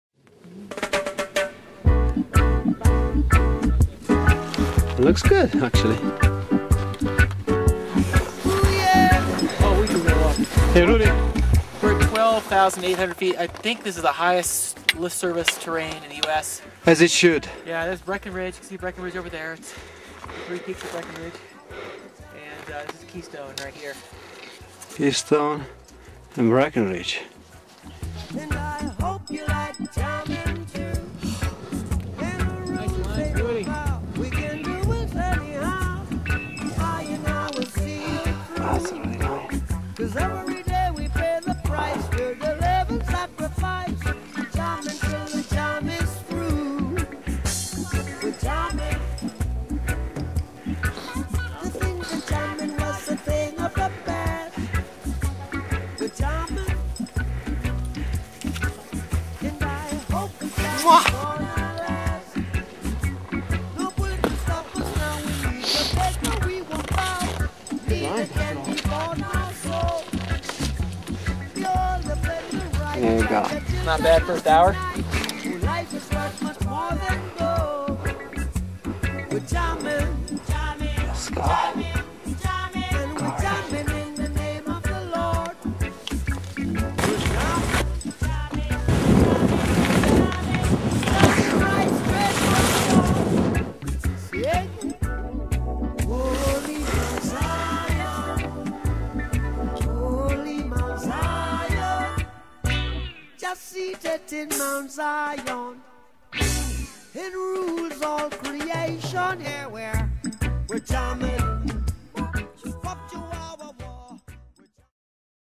Powder Skiing in the Montezuma Bowl at Arapahoe
Powder Skiing in the newly opened montezuma Bowl at Arapahoe Basin, CO